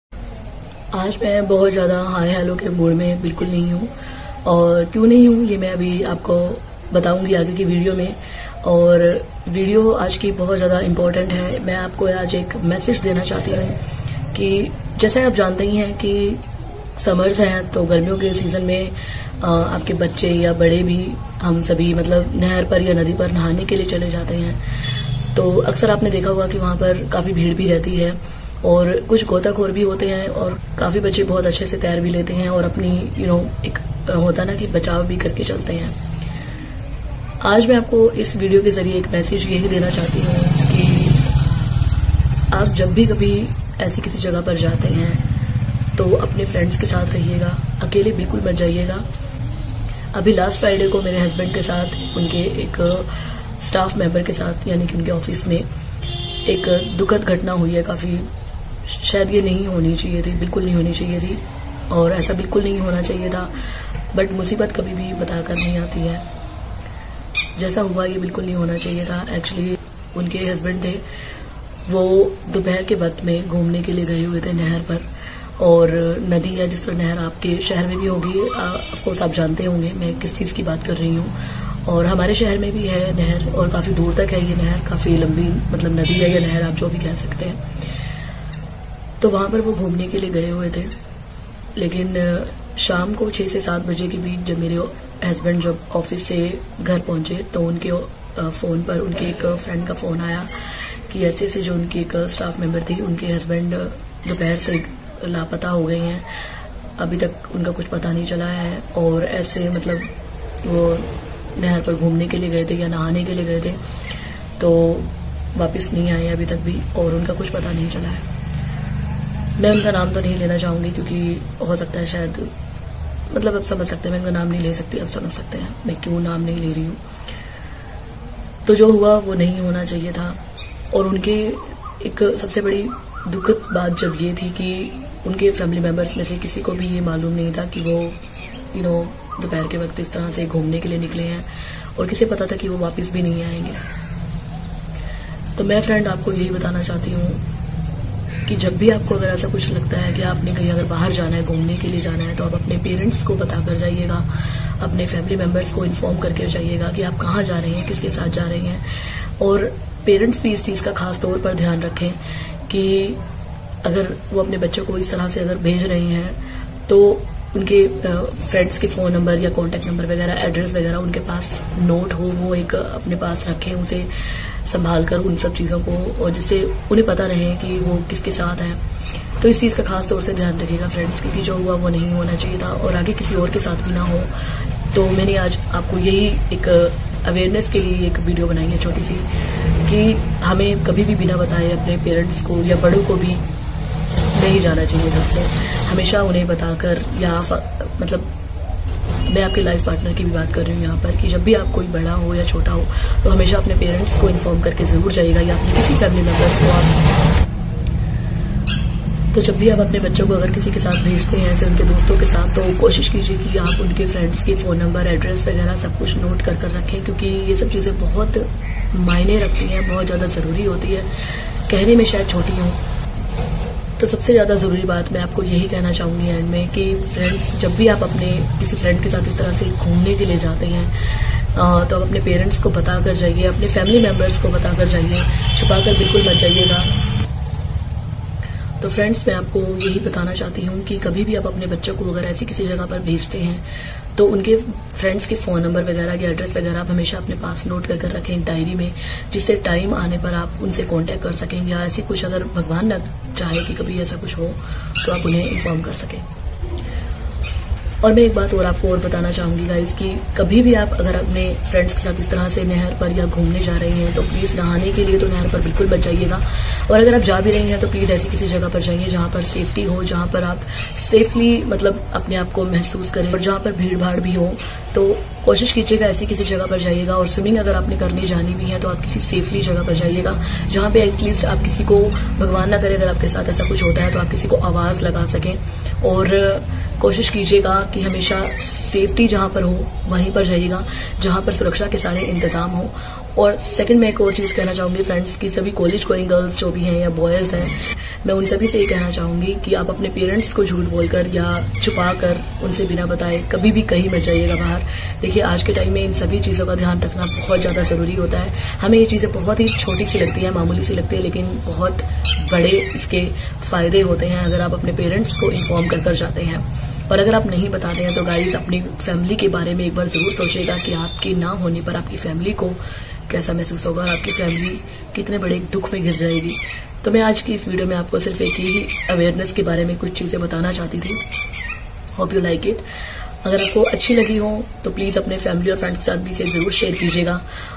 काश की वो नहर पर ना गए होते।SelfAwareness.VarshaBeautyCare.
आज मैं आपके साथ एक सचची घटना बताने जा रही हूंँ।